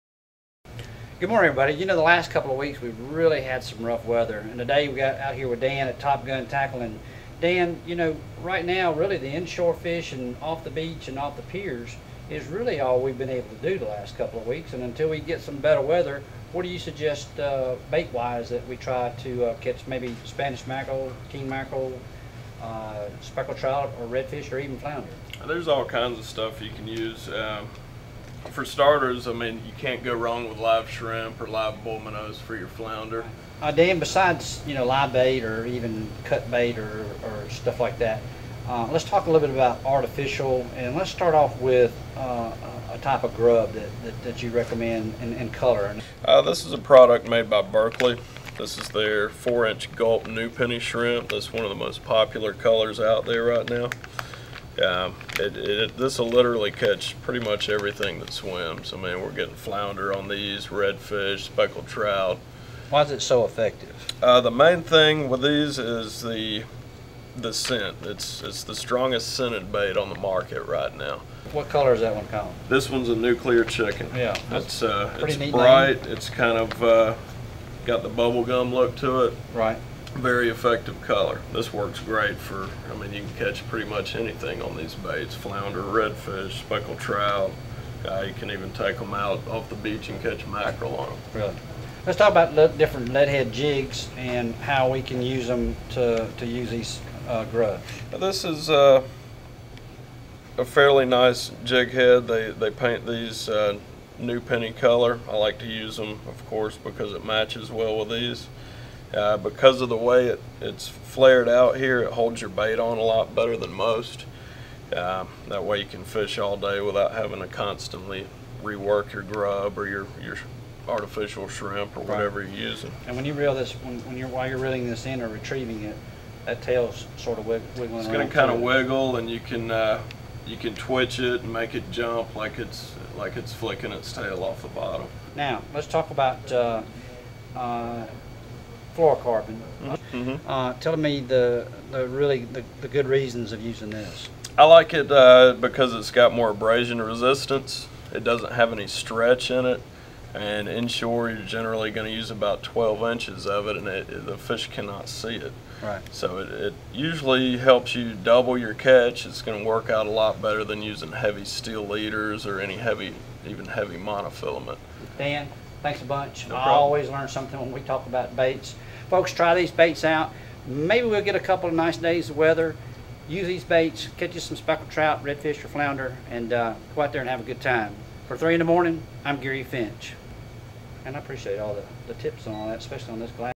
Gary Finch Outdoors-Speckled Trout Tackle
Dan Silsbee at Top Gun Tackle gives Gary advise on speckled trout baits and lures.